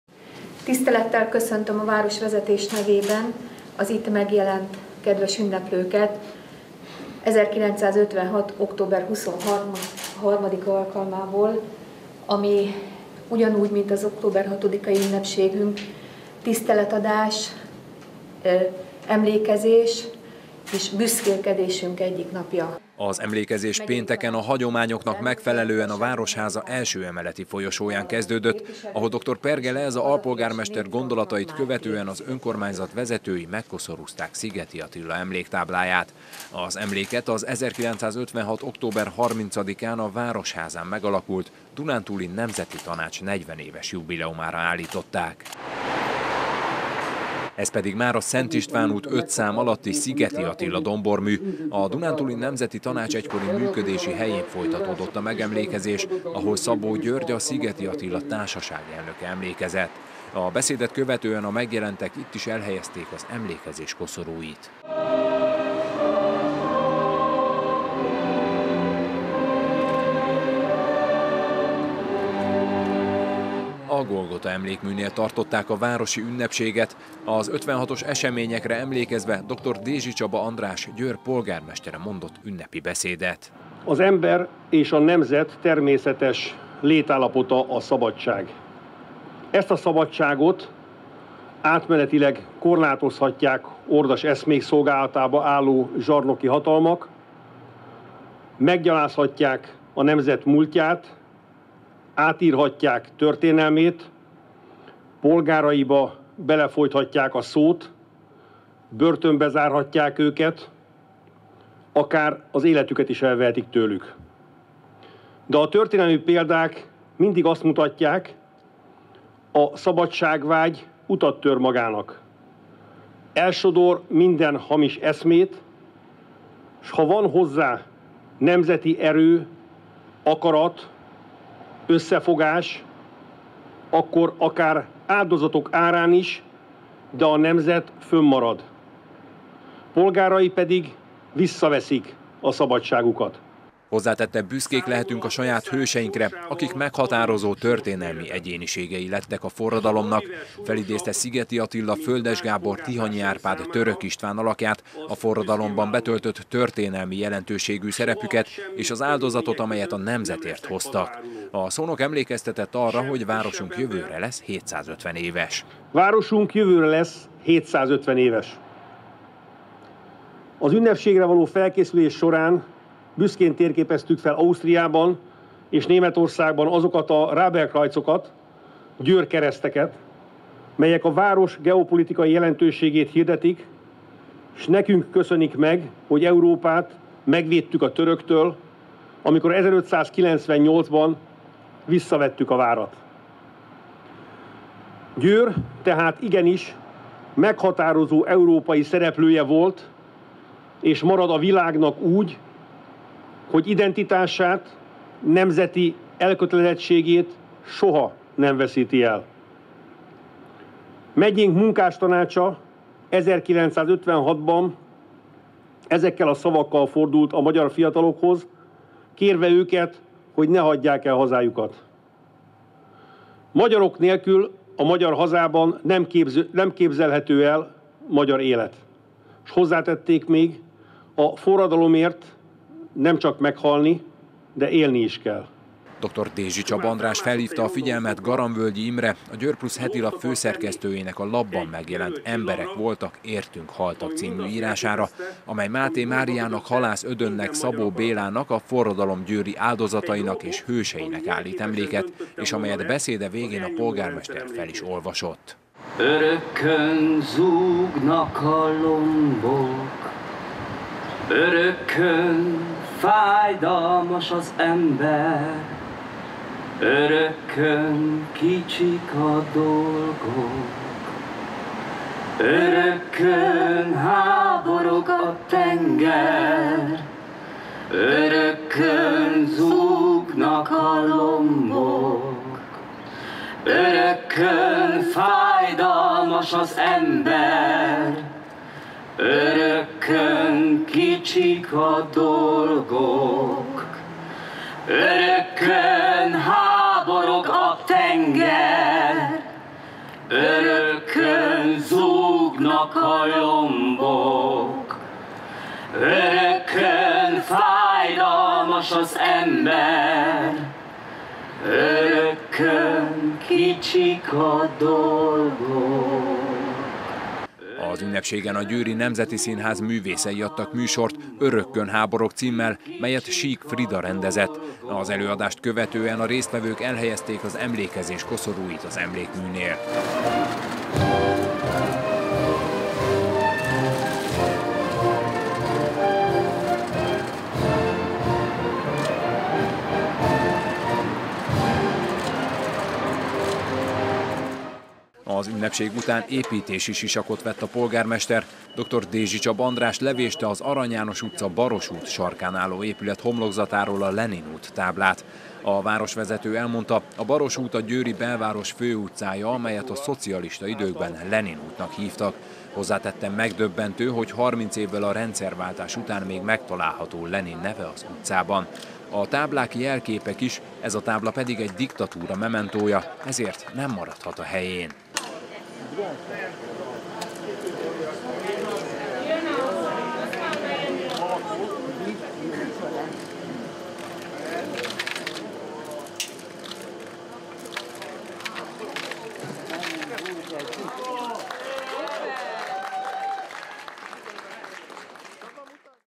"A szabadságvágy utat tört magának"
A város koszorúzási ünnepségen emlékezett az 1956-os forradalom és szabadságharc eseményeire és hőseire. Az eseményen Dr. Dézsi Csaba András mondott ünnepi beszédet. Győr polgármestere kiemelte, a történelmi példák mind azt mutatják: a szabadságvágy utat tör magának, elsodor minden hamis eszmét, s ha van hozzá nemzeti erő, akarat, összefogás, akkor, ha áldozatok árán is, de a nemzet fönnmarad, polgárai pedig visszaveszik szabadságukat. Az ünnepség után a polgármester eltávolította a „Lenin út” táblát egy Baross úti épületről.